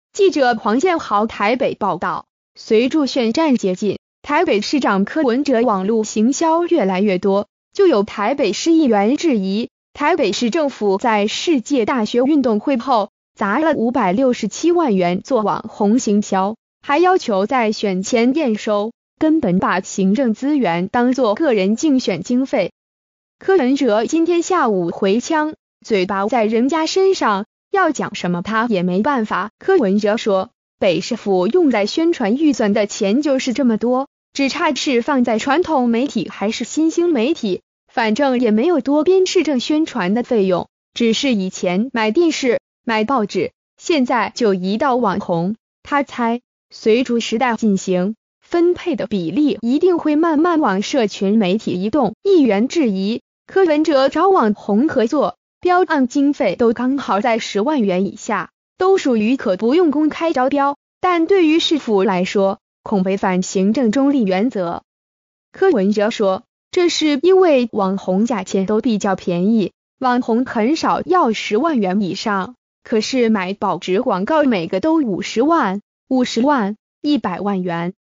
砸567萬網紅行銷挨批公帑競選 柯P回嗆：嘴巴長人家身上
〔記者黃建豪／台北報導〕隨著選戰接近，台北市長柯文哲網路行銷越來越多，就有台北市議員質疑，台北市政府在世界大學運動會後，砸了567萬元做網紅行銷，還要求在選前驗收，根本把行政資源當做個人競選經費。柯文哲今天下午回嗆，嘴巴在人家身上，要講什麼他也沒辦法。

發表評論評論如下。